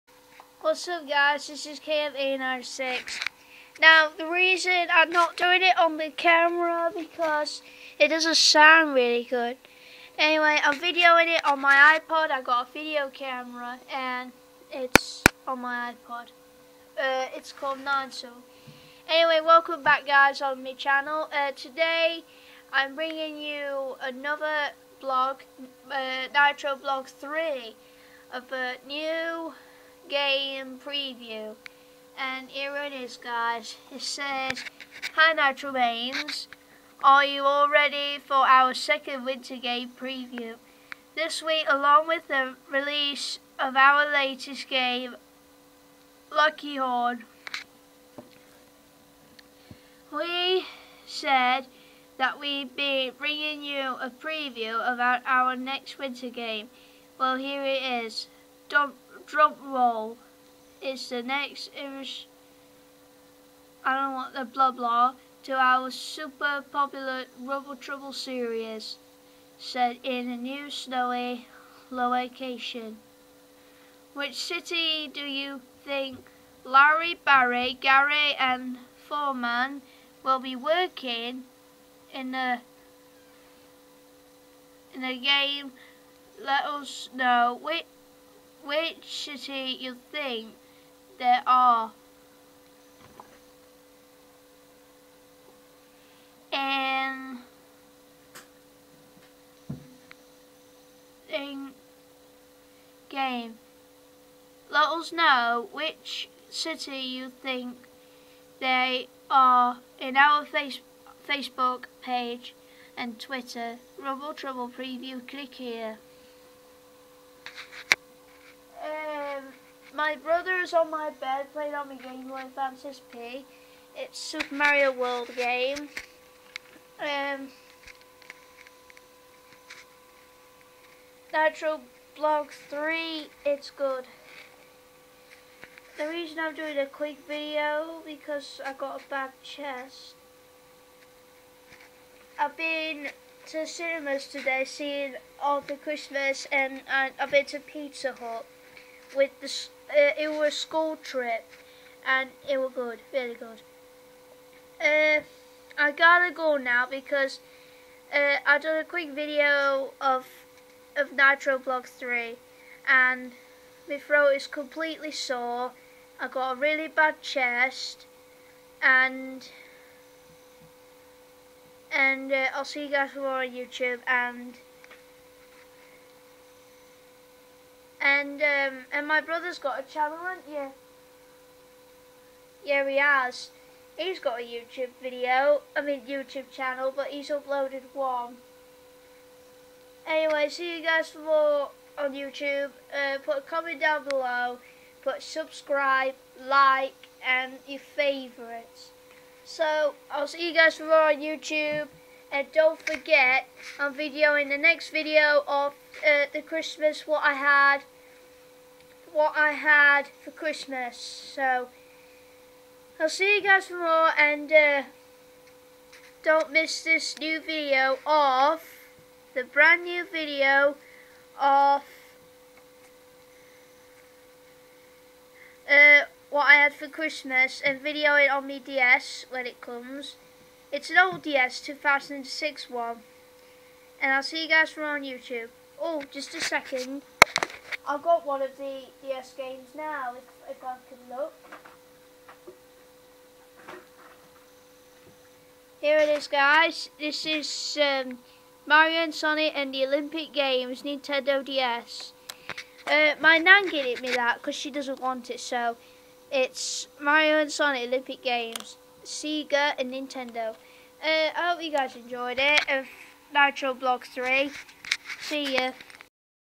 nitrome blog 3 a quick video
i made another video is nitrome blog 3 guys im a bit ill tonigth guys i realy got a bad chest and i hope i get better soon when i was making a video my brother was on my bed playing my gameboy sp on super mario world see you later